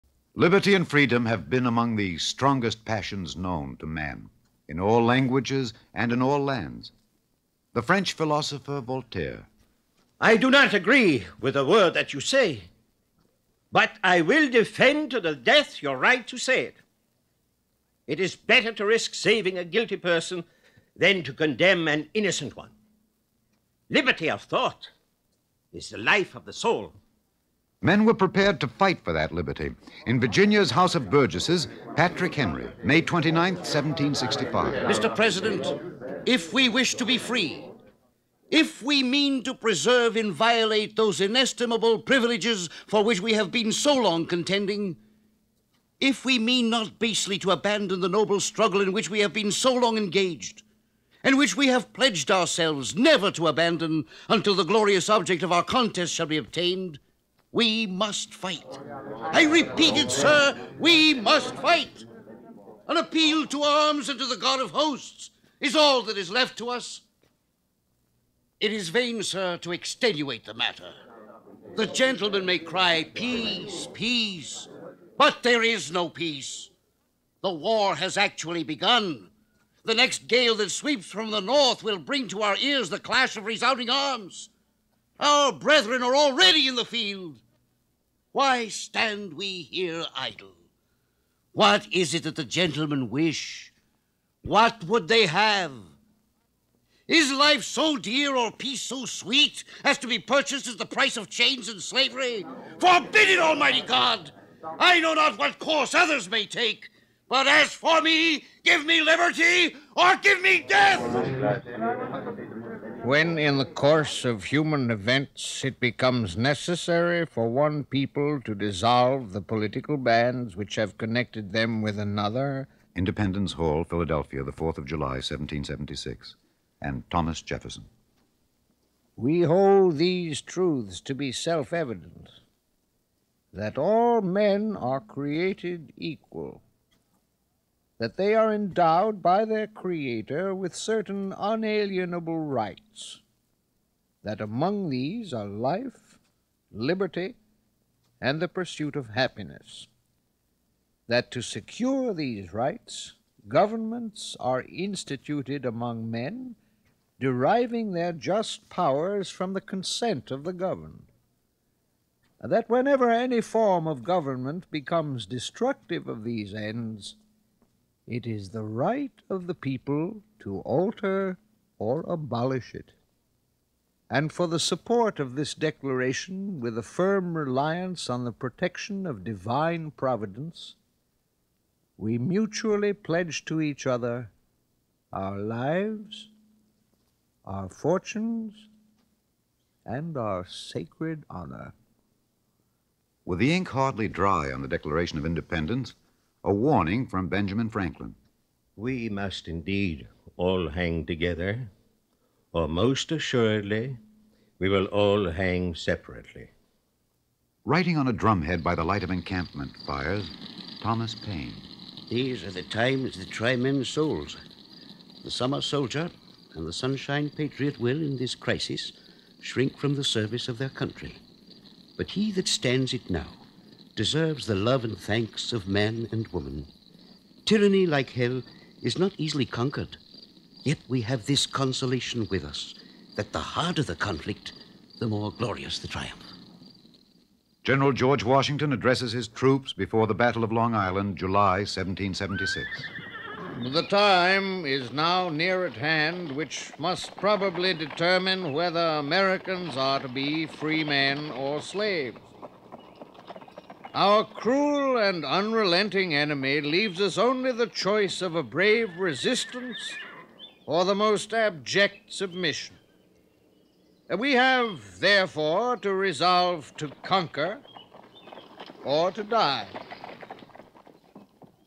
The Words Of: Voltaire, Patrick Henry, Jefferson, Franklin, Paine, Washington
The Words Of: Voltaire, Patrick Henry, Jefferson, Franklin, Paine, Washington · Joseph Wershba

Great Moments of Oratory